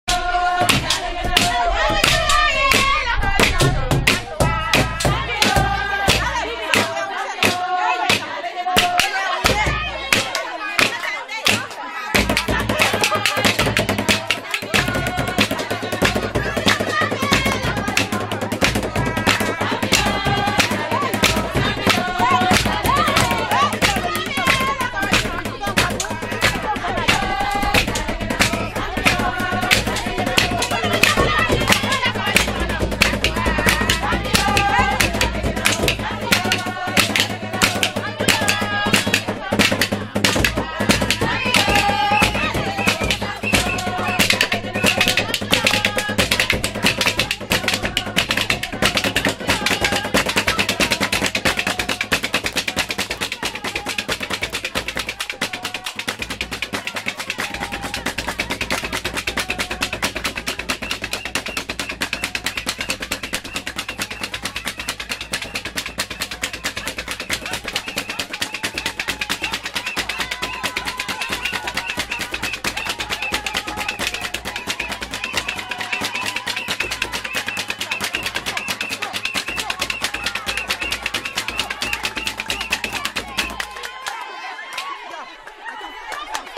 Naming Ceremony, Senegal / 세네갈 명명식2
[세상의 끝에서 만난 음악] 103쪽 - "밥보다 춤"
1주일 전에 태어난 아기의 명명식이 끝나고 여자들만 모여 춤추고 노래하며 피로연을 갖는다. 네 시간 정도 모여 노는데 놀이의 핵심은 온몸을 맹렬하게 흔드는 춤을 추며 누가 더 오래 출 수 있는지 대결을 벌이는 것이다.

Place: Diakenwolof, Casamence, Senegal
Date: Mar, 10, 2017
Recorded by Sangil Choi